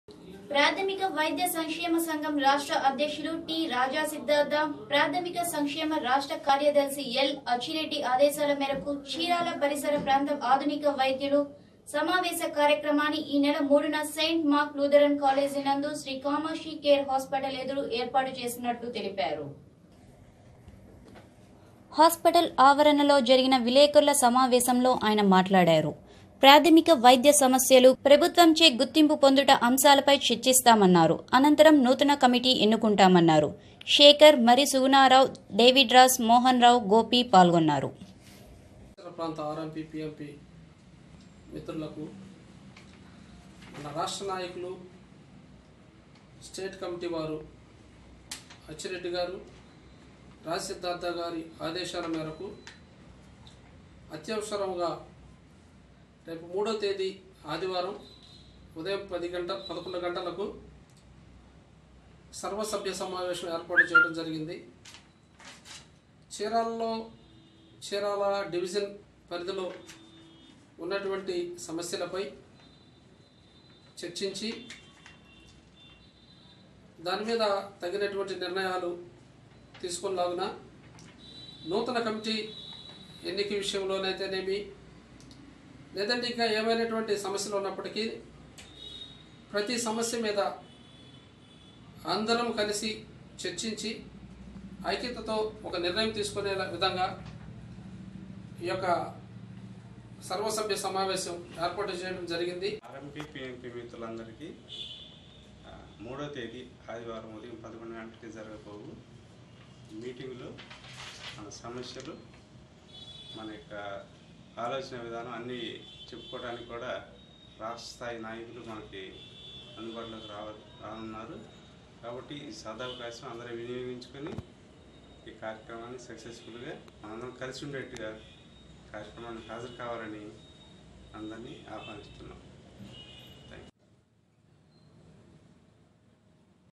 మార్చి 3న చీరాలలో గ్రామీణ ప్రాధమిక వైద్యుల సమావేశం || Dn5
ప్రాథమిక వైద్య సంక్షేమ సంఘం రాష్ట్ర అధ్యక్షులు టి రాజా సిద్ధార్థ్, ప్రాథమిక సంక్షేమ రాష్ట్ర కార్యదర్శి ఎల్ ఆచ్చిరెడ్డి ఆదేశాల మేరకు చీరాల పరిసర ప్రాంత ఆధునిక వైద్యులు సమావేశ కార్యక్రమాన్ని ఈ నెల 3న సెయింట్ మాక్స్ లూధరిన్ కాలేజ్ నందు శ్రీ కామాక్షి కేర్ హాస్పిటల్ ఎదురు ఏర్పాటు చేసినట్లు తెలిపారు.
హాస్పిటల్ ఆవరణలో జరిగిన విలేకరుల సమావేశంలో ఆయన మాట్లాడారు. ప్రాథమిక వైద్య సమస్యలు, ప్రభుత్వంచే గుర్తింపు పొందుట అంశాలపై చర్చిస్తామన్నారు. అనంతరం నూతన కమిటీ ఎన్నుకుంటామన్నారు. శేఖర్, మరి సుగుణరావు, డేవిడ్ రాజు, మోహన్ రావు, గోపి పాల్గొన్నారు.